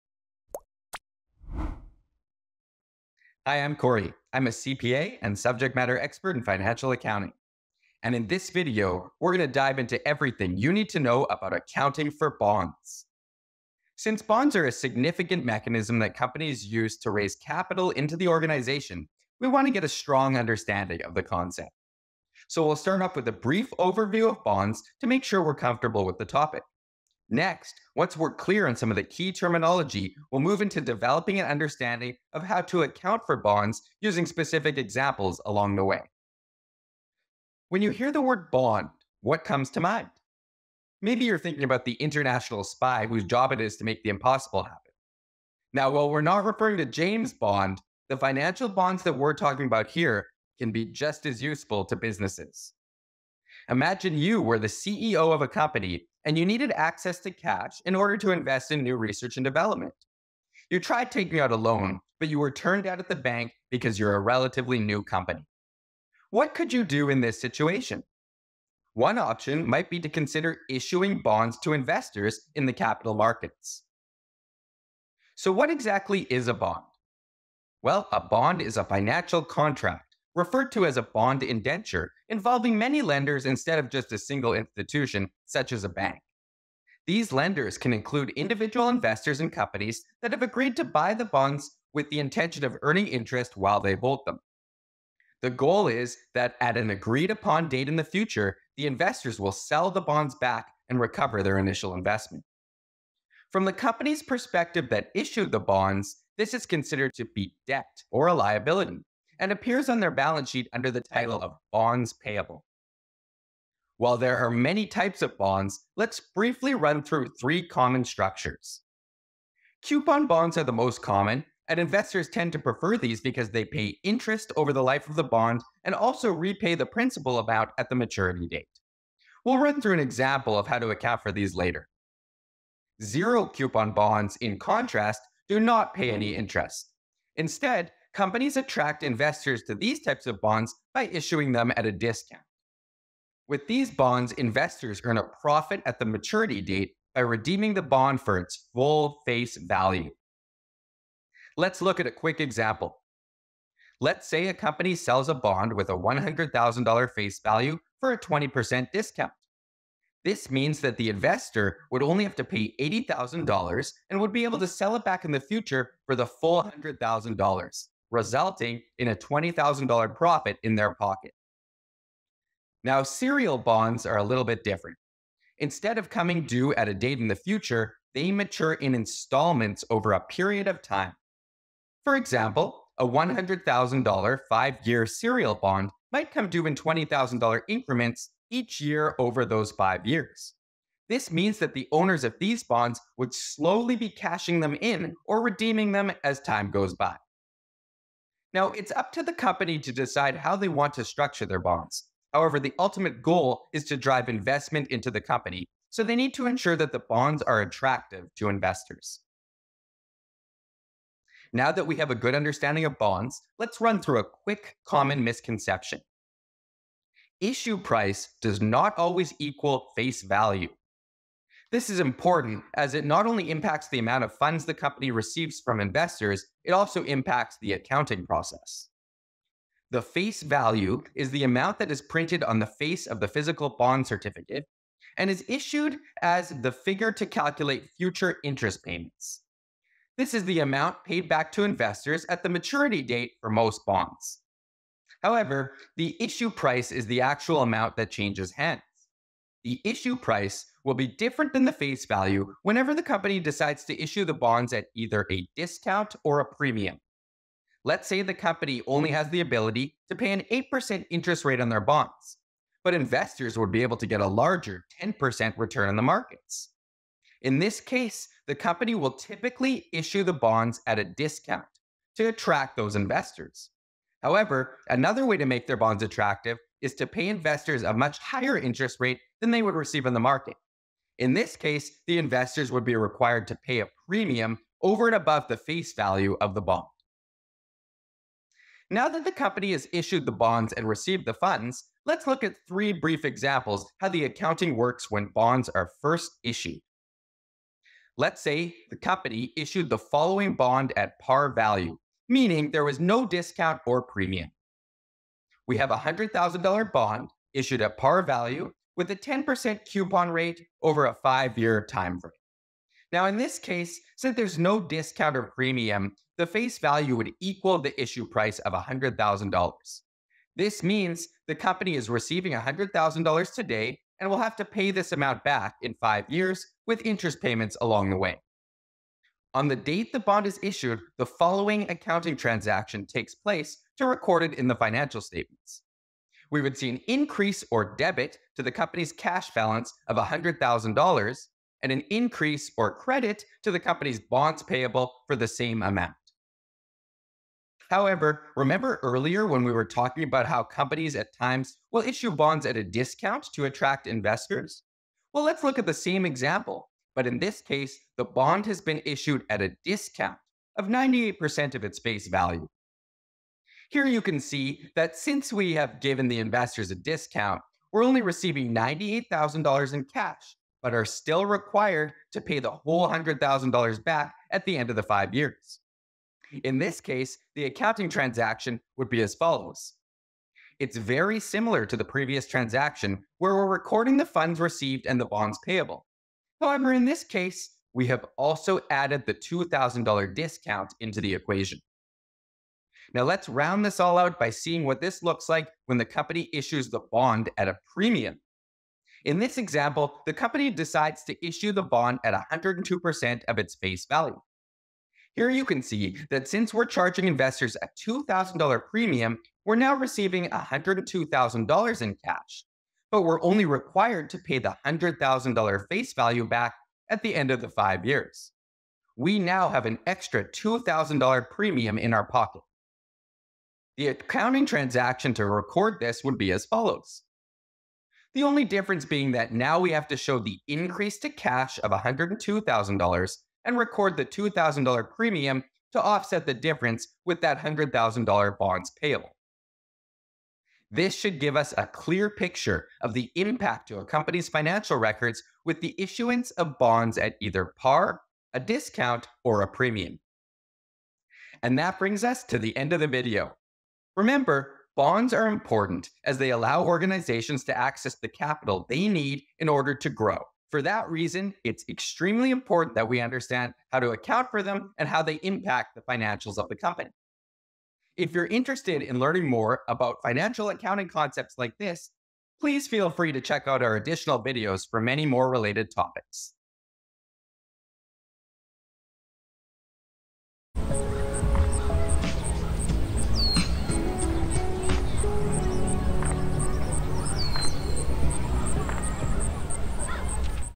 Accounting For Bonds | Financial Accounting
Bonds are a significant mechanism that companies use to raise capital into their organizations. We'll learn what bonds are and their key terminology (including coupon bonds, zero coupon bonds, and serial bonds), then get into how to account for bonds.

00:00 Intro
00:41 What are bonds?
02:14 Bond structures
3:42 Common mistakes
04:54 Examples
07:31 Recap